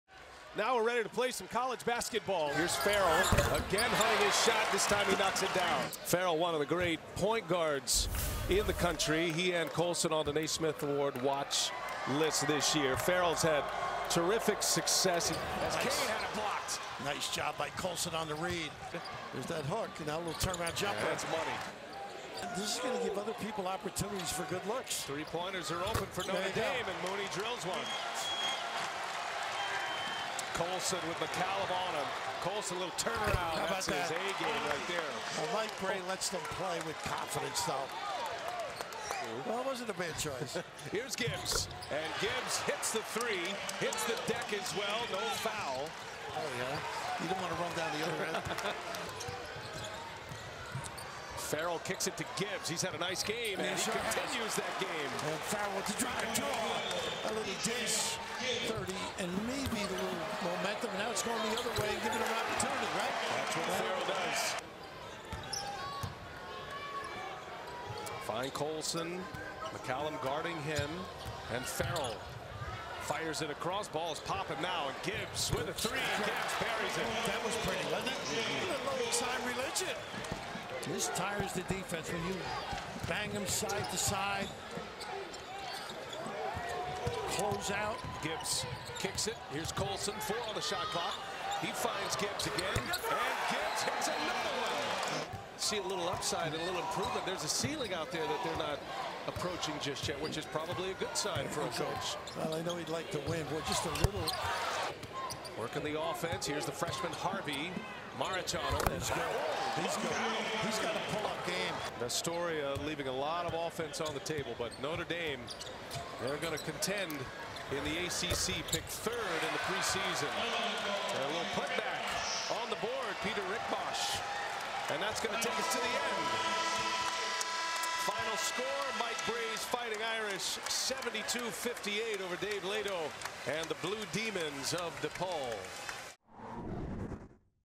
Highlights | @NDmbb at DePaul (2017)
Highlights from Notre Dame's 72-58 victory over DePaul.